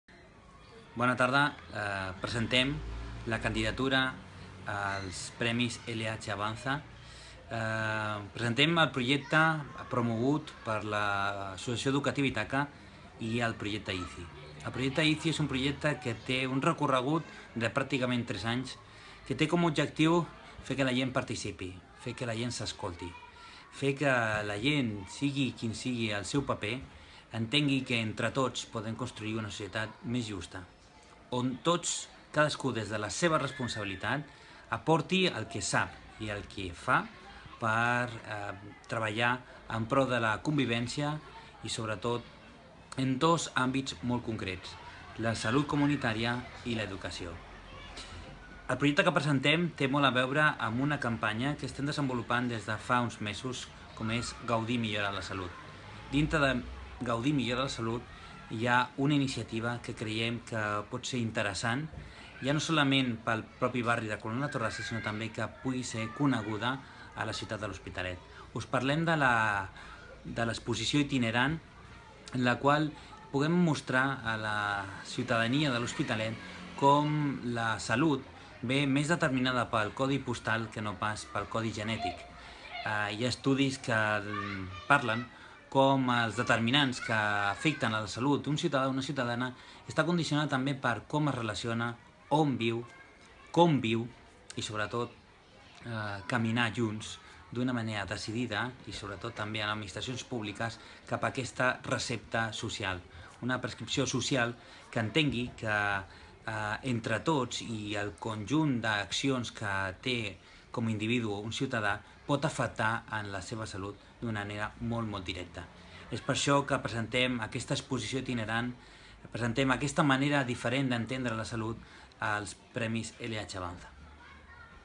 Associació Educativa Itaca. El Buen Vivir
Des del convenciment que la salut comunitària és una de les eines més valuoses per treballar cap el benestar individual i col·lectiu, la línia de salut comunitària és una de les línies prioritàries del treball comunitari que es desenvolupa a Collblanc - la Torrassa en el marc del Projecte d’Intervenció Comunitària Intercultural (Projecte ICI).

Els/les professionals de la Taula de Salut Comunitària s’han proposat promoure un concepte de salut holística amb la voluntat de contribuir a la millora de la salut dels veïns i veïnes de Collblanc – la Torrassa.

Per fer-ho s’ha impulsat la Campanya Gaudir millora la salut: Cicle d’iniciatives adreçades al conjunt de la ciutadania on la cultura i els hàbits saludables són les eines per difondre el concepte de salut integral i generar espais de relacions. Així, es vol contribuir a la millora de la salut comunitària.

En el marc d’aquesta Campanya sorgeix l’exposició “El buen vivir”, projecte que presentem a LH AVANÇA amb l’objectiu de fer arribar l’exposició al major nombre de persones ciutadanes de la ciutat de L’Hospitalet.

Sovint hi ha la creença que la genètica determina la salut de les persones. Des d’aquesta perspectiva, mantenir un bon estat de salut dependria, fonamentalment, de les possibilitats d’accedir a un sistema sanitari avançat.

No obstant, existeixen altres factors que determinen la nostra salut i ens permeten viure bé.

Com especifica l’Agència de Salut Pública de Catalunya), l’80% dels determinants de la nostra salut es troben fora de l'àmbit sanitari i tenen un origen social o ambiental. Solament el 20% restant correspon a factors genètics o biològics (dades de l’Agència de Salut Pública de Catalunya).

Factors propis de la persona i del seu entorn com poden ser l'estil de vida, les xarxes de relacions, les condicions laborals i d'habitatge, la cura de l'infant durant els primers anys de vida, l’accés a l'educació, la cultura i els espais verds.

Aquests elements determinen la salut de les persones. Ser conscients d'aquesta realitat i actuar en conseqüència, obre la possibilitat a millorar ostensiblement el nostre benestar.

L’exposició es proposa com a espai on podem informar-nos i reflexionar sobre els diferents factors socials que influeixen en la salut i conèixer els diferents serveis del territori que contribueixen en la seva millora. És una crida a la responsabilitat vers la pròpia salut i dels altres. Som corresponsables en la construcció d’una comunitat sana i feliç.

L’exposició ha estat activa al llarg d’un mes al centre cultural del districte de Collblanc – la Torrassa i ara, el següent pas, es poder aconseguir que l’exposició sigui itinerant i pugui estar present en els diferents districtes de la ciutat.

Tenint en compte que Segons l'Organització Mundial de la Salut (OMS), la salut és un estat de complet benestar físic, mental i social. Creiem important que les persones prenguin consciència que la salut no només és absència de malaltia o feblesa i és per això que volem sensibilitzar i conscienciar a la població d’Hospitalet fent partícip i accessible l’exposició a tota  la ciutadania de la ciutat de l’Hospitalet.

Per fer el seguiment del projecte es valoraran indicadors de participació tenint en compte no únicament el volum de persones ciutadanes que assisteixen sinó que també si aquestes utilitzen els mitjans d’interacció habilitats. Trobem, entre d’altres, la possibilitat de deixar constància del que els hi ha semblat l’exposició i de compartir allò que ells i elles entenen per salut.

A més a més, també es tindrà en compte si ha agut impacte mediàtic en les diferents inauguracions de l’exposició en els diferents districtes, si altres entitats s’han sumat arrel de l’existència i coneixement de la iniciativa...